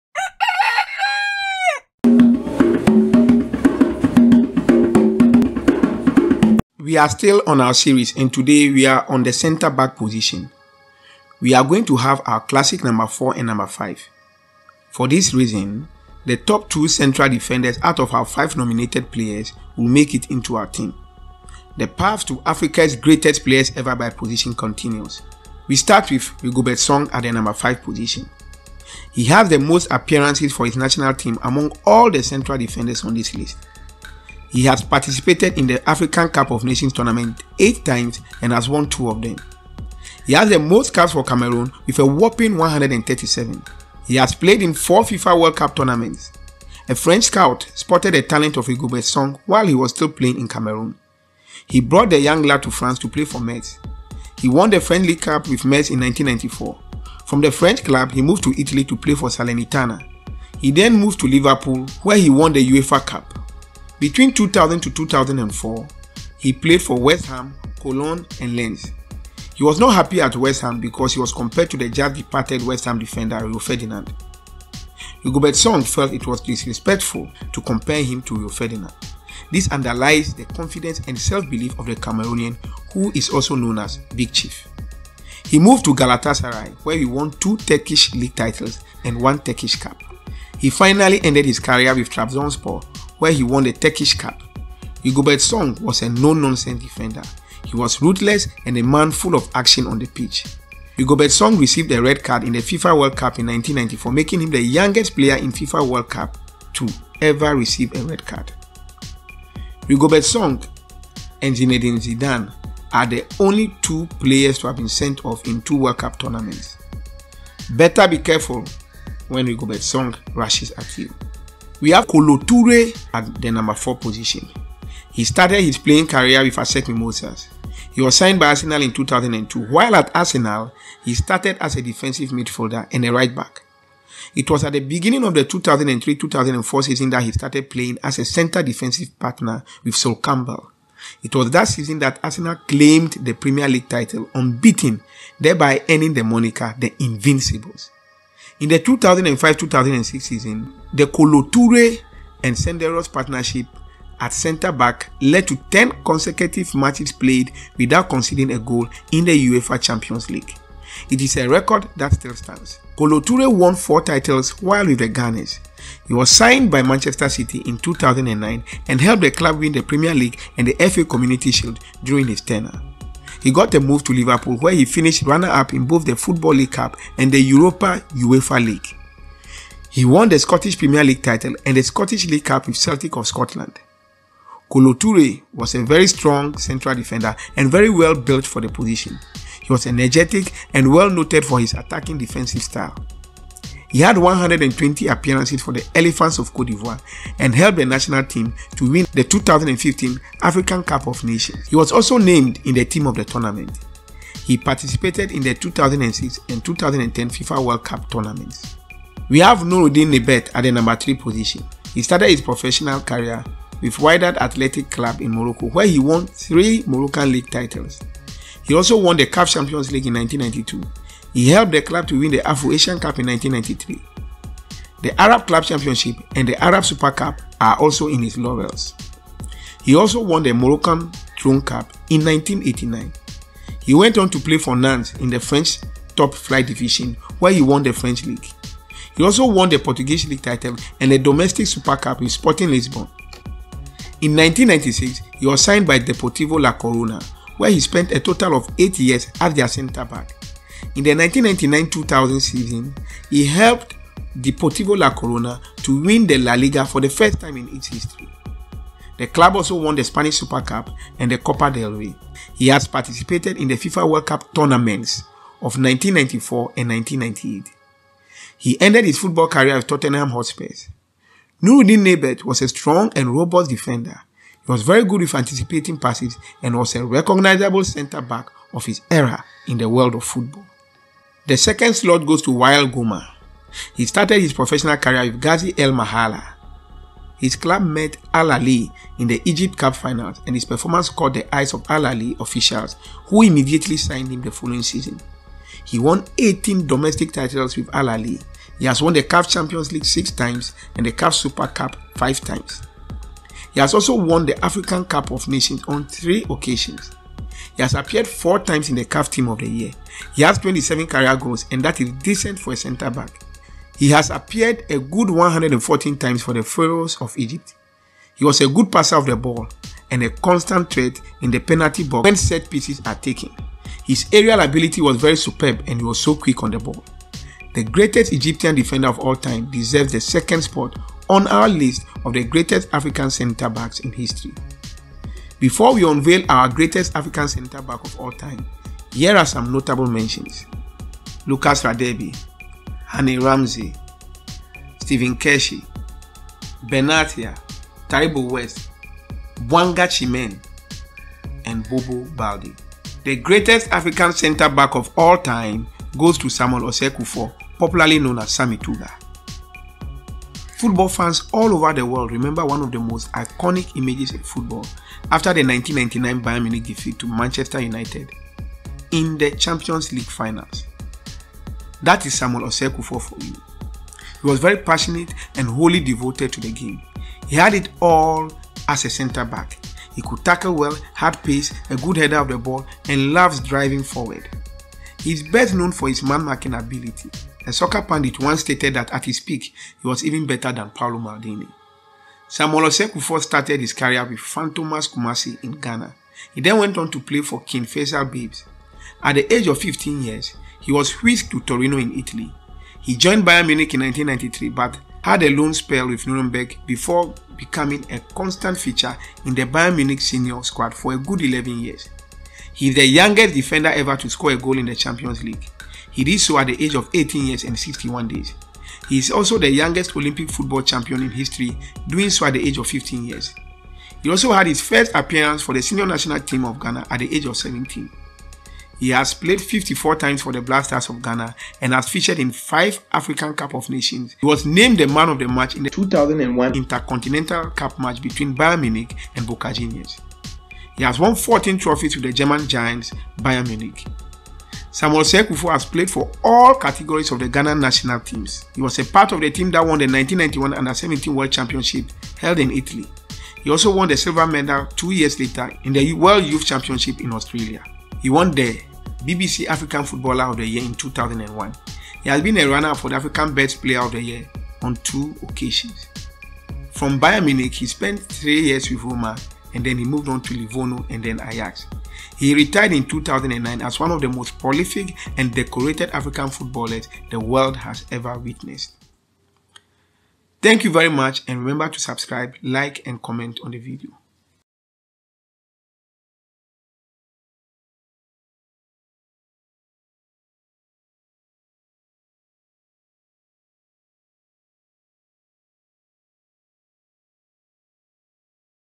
The 5 Greatest African Center- Backs of All Time - The Road to Africa's Greatest XI in History.
The center-back position is a very key position in football as they are the last line of defense in football. Africa has produced many talents in this position. For this video, the players that attain the number one and number two positions will be included in our list of the greatest African players of all time by position. Th

We have also made some honourable mentions along the way.